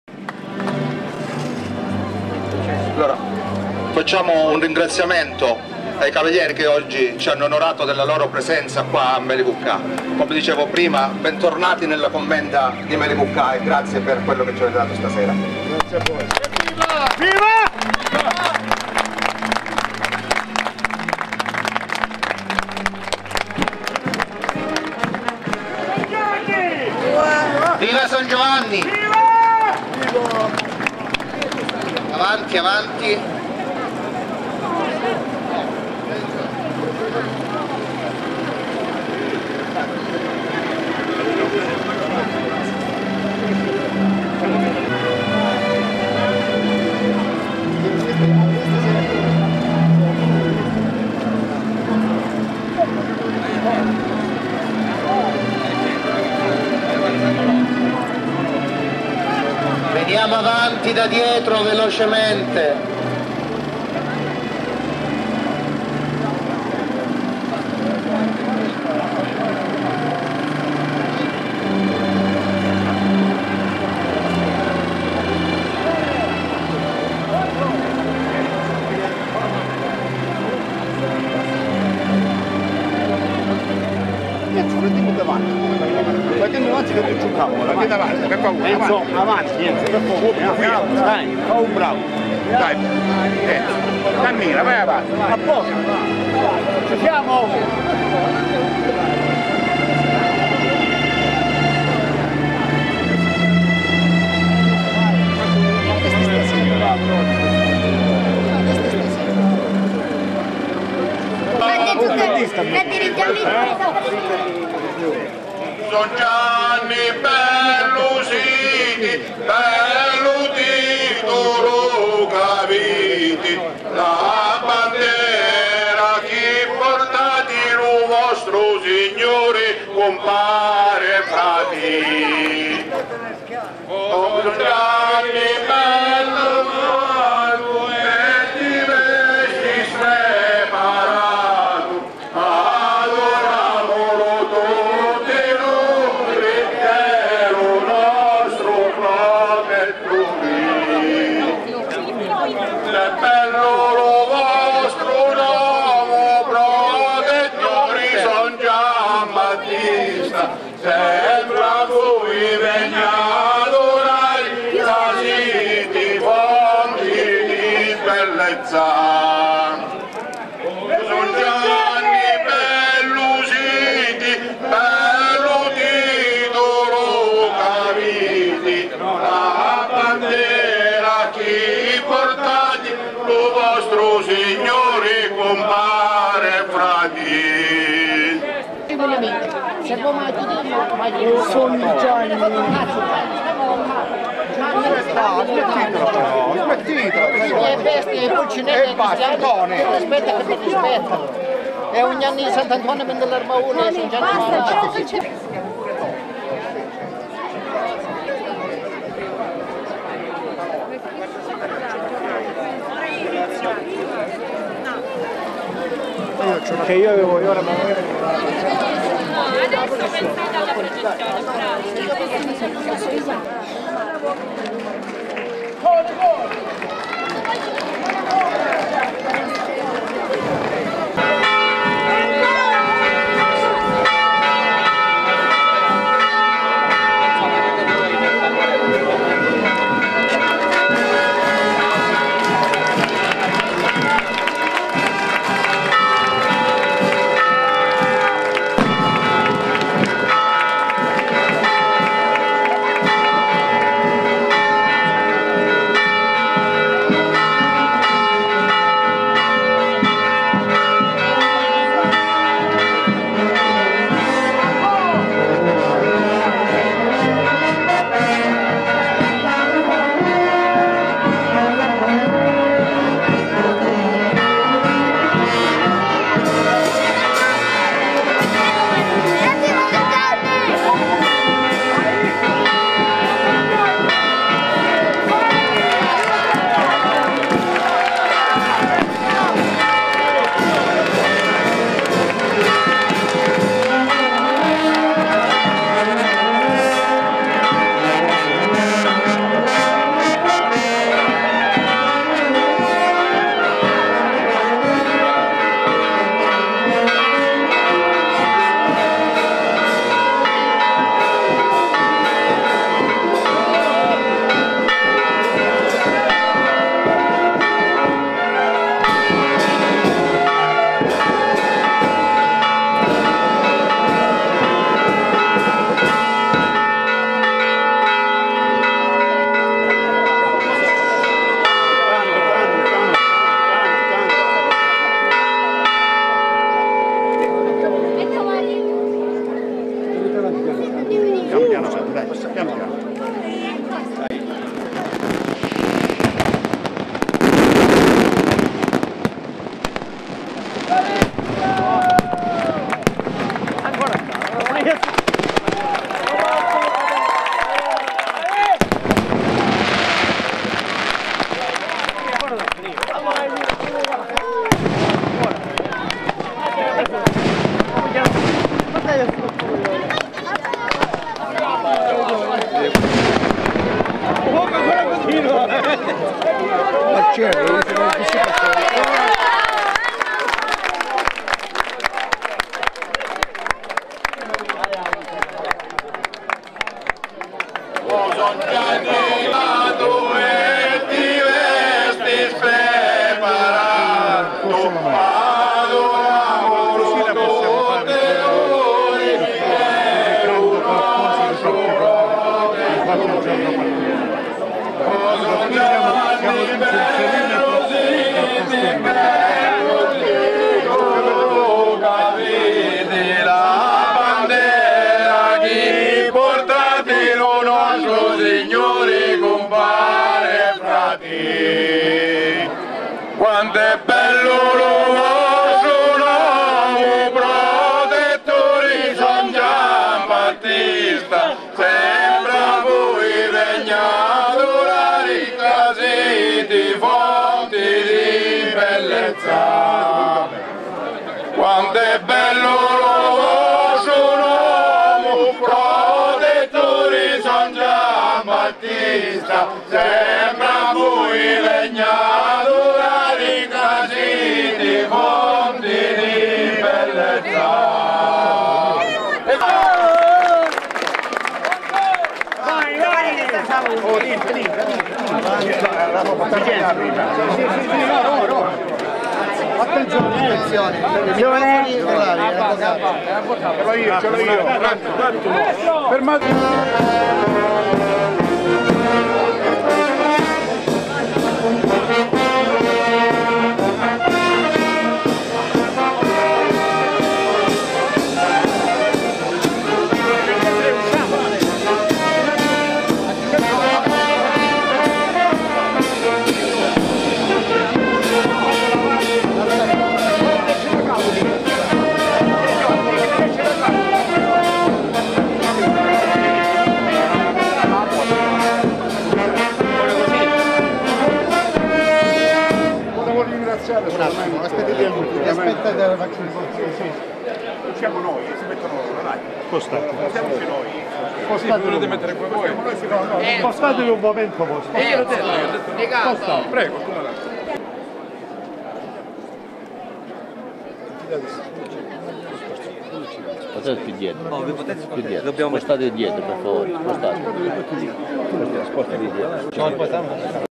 MELICUCCA' SAN GIOVANNI BATTISTA 2013 DI GIOVANNI TOMARCHIO 7 DA 8 ]
SAN GIOVANNI BATTISTA. Ogni anno il paese rivive momenti coagulanti di vita civile e religiosa per onorare il suo Patrono S. Giovanni battista. Due giornate il 23 e il 24 giugno che sono per i melicucchesi motivo di vanto e di orgoglio, in quanto rappresentano un momento di aggregazione della comunità che, seppure colpita e provata nel corso dei secoli, è sempre pronta, con rinnovata vitalità, a raccogliersi attorno al proprio protettore. Il culto e la venerazione per S. Giovanni
Oggi, il popolo di Melicuccà, ripete con intensa devozione l'antico rituale, portando in corteo un quadro di S.Giovanni dalla Chiesa fino alla Torre Civica